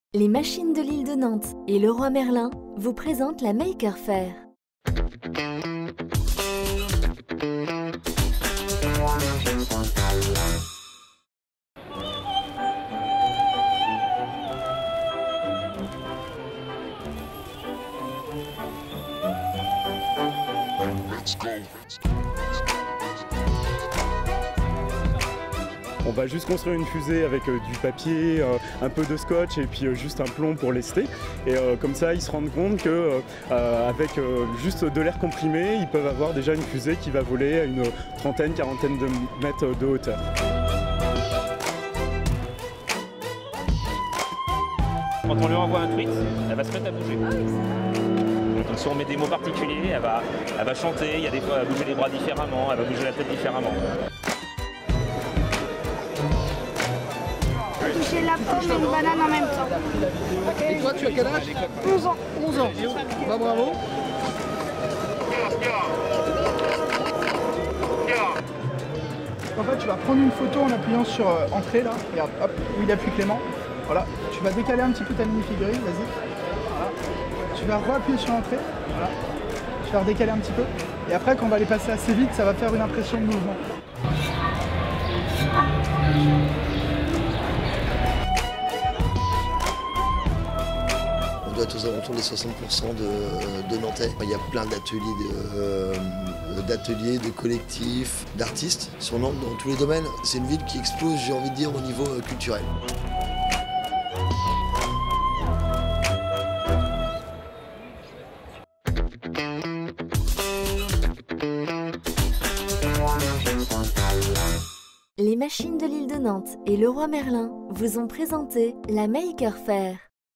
Les makers : jour 5
Ils jouent à domicile... 60% des Makers présents cette année viennent de Loire-Atlantique. Particuliers, entreprises, écoles, le dynamisme créatif nantais est plus que jamais d'actualité. Un ancrage local qui ravit les organisateurs.